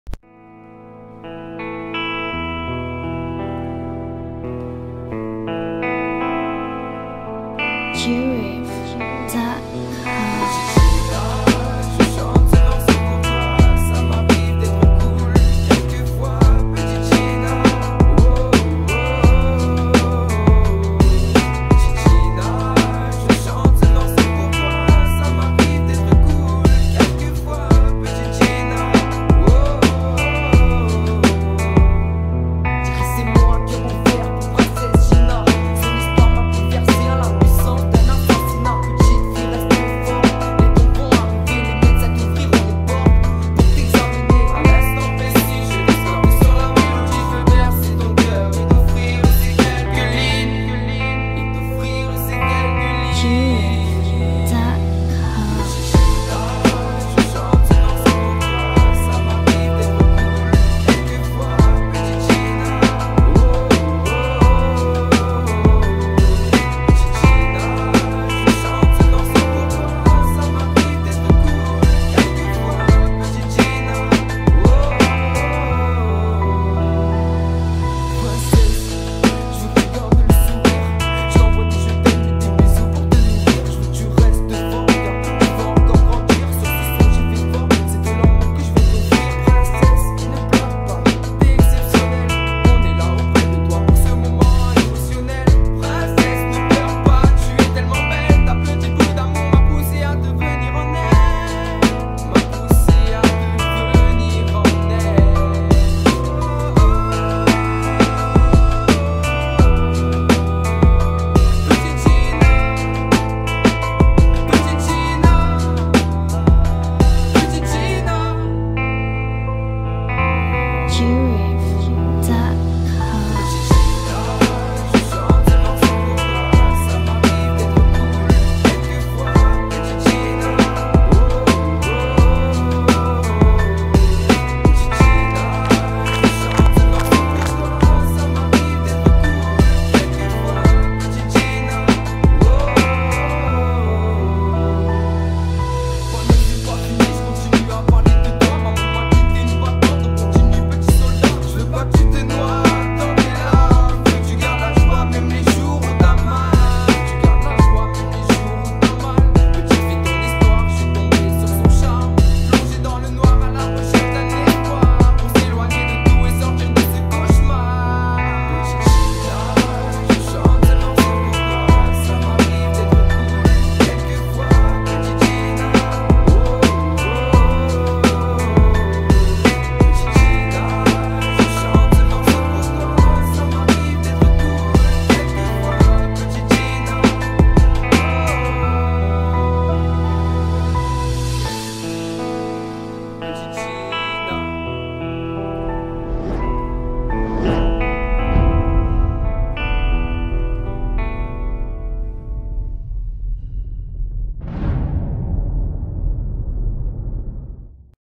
Gina, son 23 MAI
Vidéo souvenir de la journée pour Princesse Gina à Le Portel Plage.